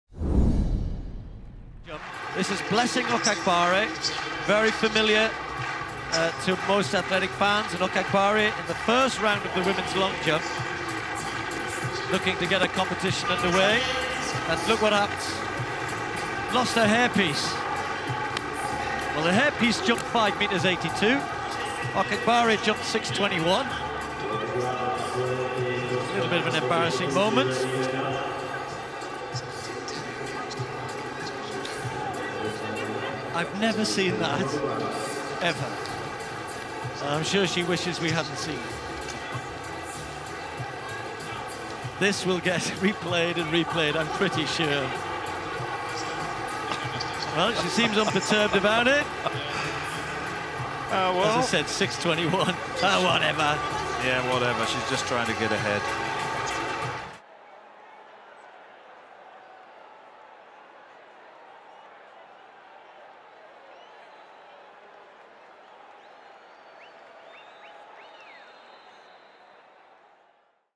Blessing Okagbare drops her hair in the Women's Long jump - IAAF Diamond League Oslo 2017
Blessing Okagbare of Nigeria had a bit of an accident during her first long jump attempt.